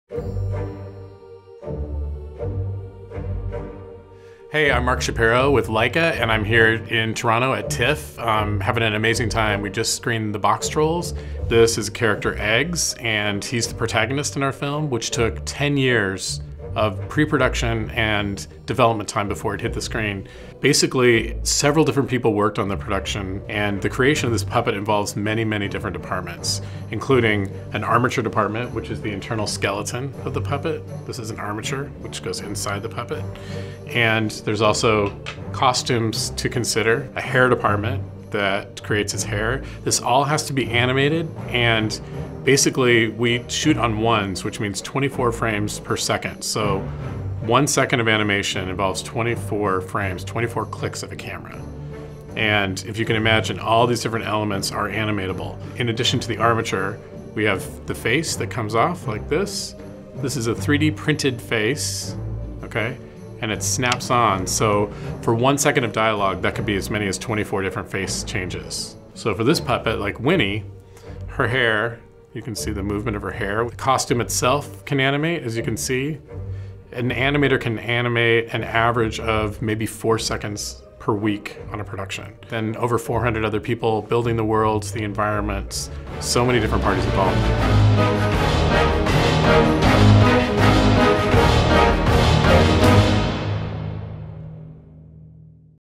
SHOW AND TELL | Mark Shapiro | TIFF 2016
Laika Animation's Head of Marketing introduces us to a couple character puppets from the Boxtrolls, Laika's most ambitious stop-motion feature to date.